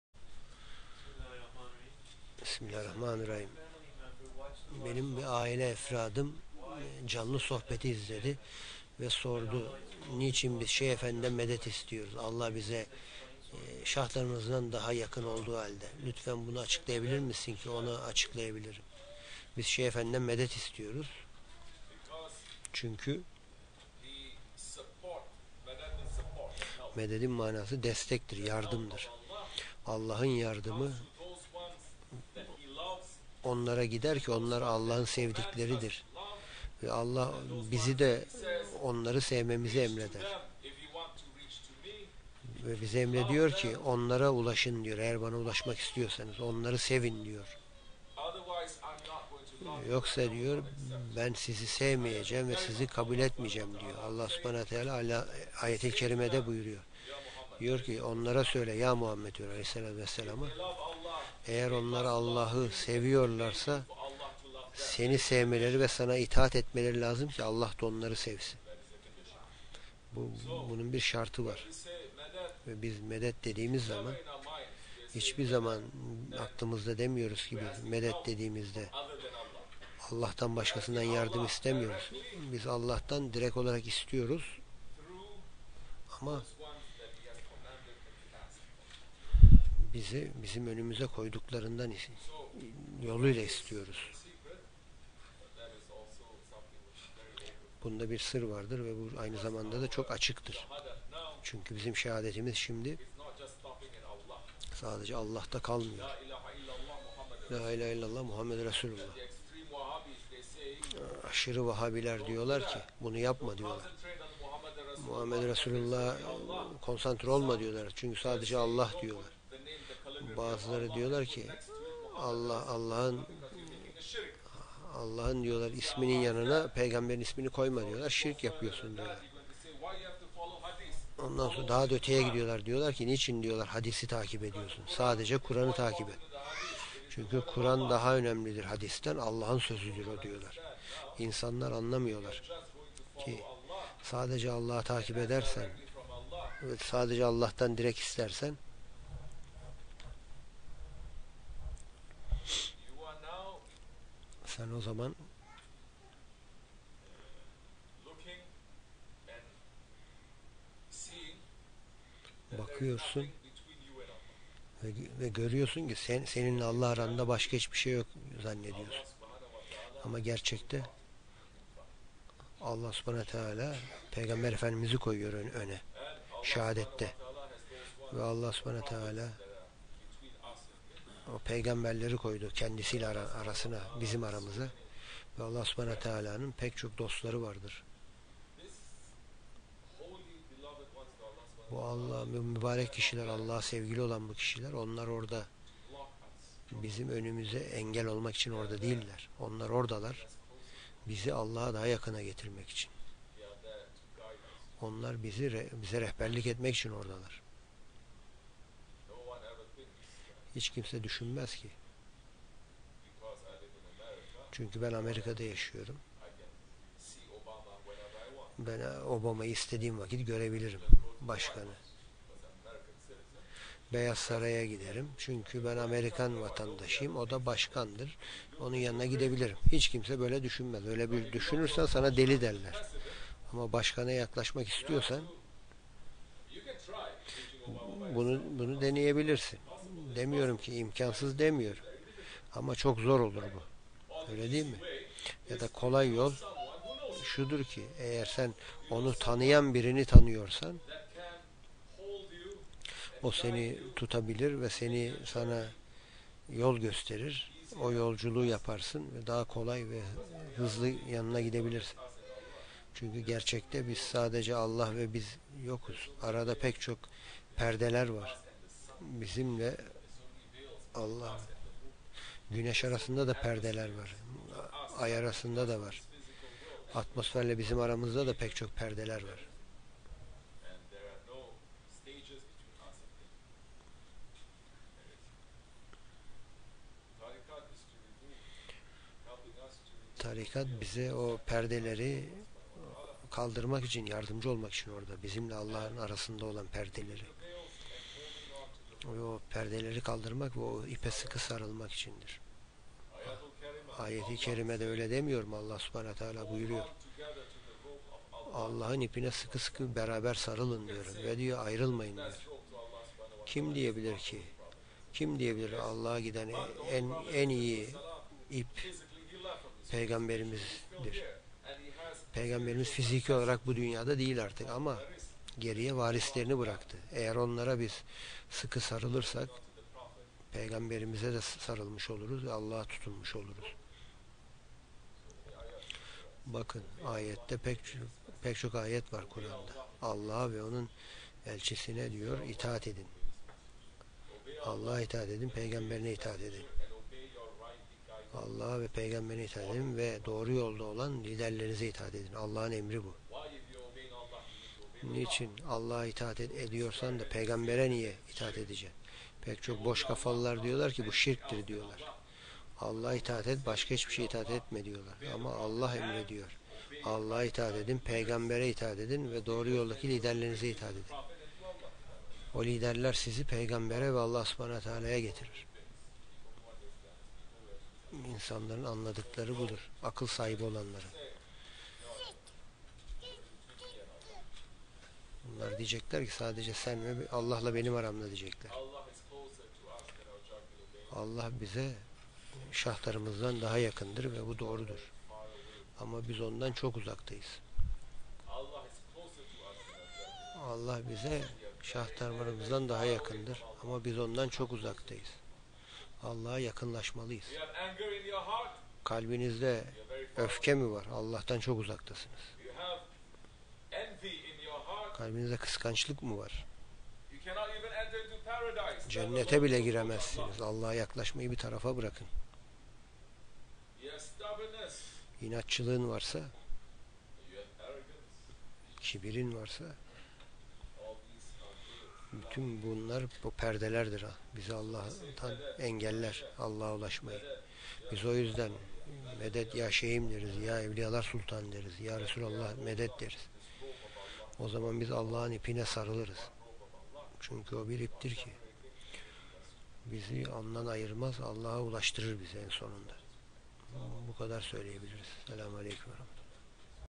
Soru: Allah bize şah damarımızdan yakınken niye Evliyadan medet isteriz?
26 Aralık 2013

* Şeyh Abdulkerim el Kıbrısi Hazretlerinin halifesi Lokman Efendi (Türkçe seslendirme)

OSMANLI DERGAHI - New York *